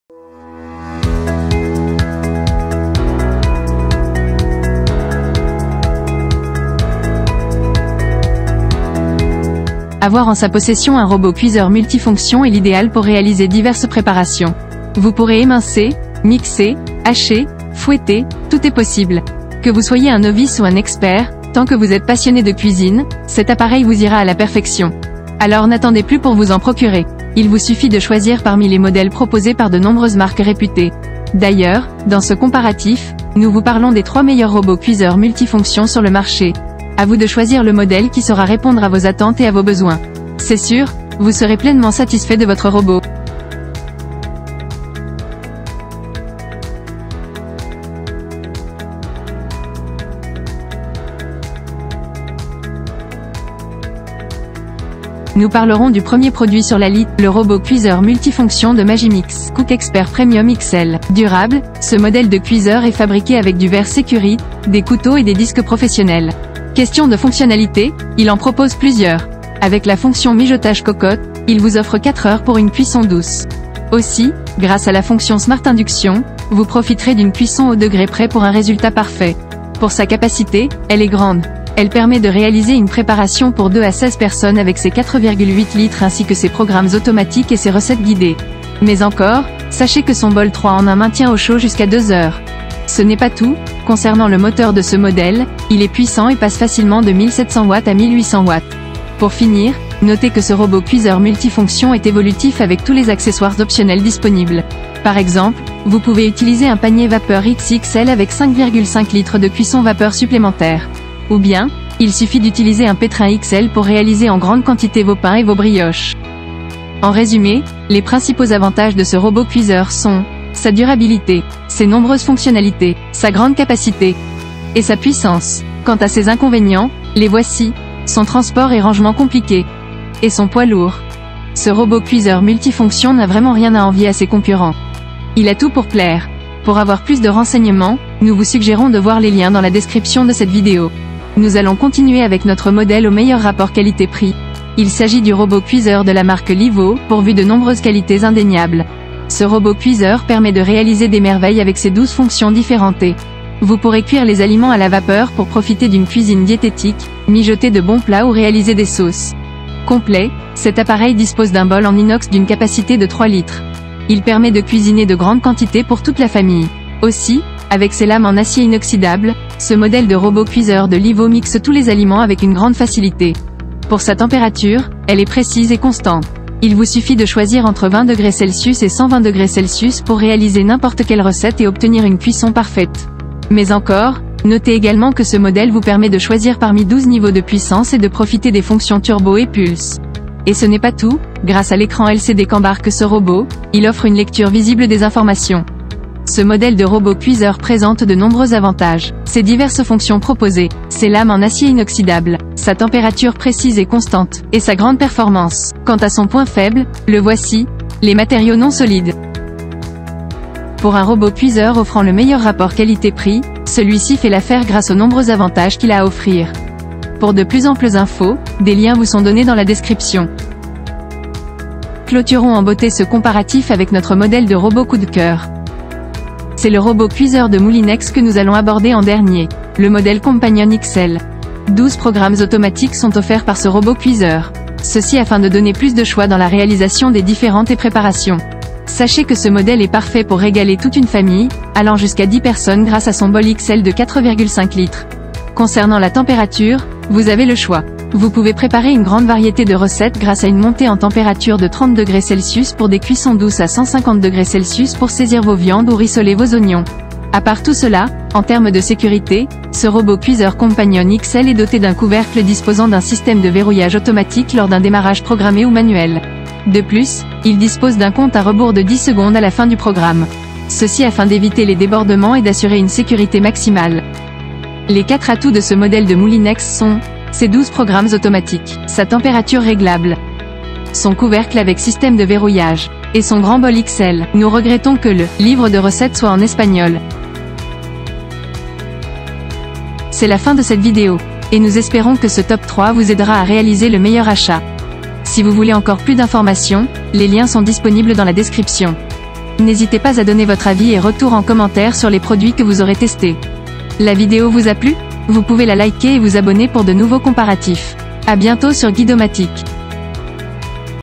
⭐️ TOP 3 : ROBOT CUISEUR MULTIFONCTION 2023
Plus d'informations sur notre top 3 👇
🥇  Robot Cuiseur Multifonction Magimix Cook Expert Premium XL
🥈 Robot Cuiseur Multifonction Livoo
🥉  Robot Cuiseur Multifonction Moulinex Companion XL

💭Si vous êtes à la recherche d'un Robot Cuiseur Multifonction, alors vous êtes sur la bonne vidéo !

🔎 Plus d'infos sur les meilleurs Robots Cuiseurs Multifonctions du moment 🔍 :

👉 Sommaire de la vidéo :

00:00 Présentation des meilleurs Robots Cuiseurs Multifonctions
00:56 Le Robot Cuiseur Multifonction Magimix Cook Expert Premium XL
02:22 Avis sur le Robot Cuiseur Multifonction Livoo
03:42 Robot Cuiseur Multifonction Moulinex Companion XL : test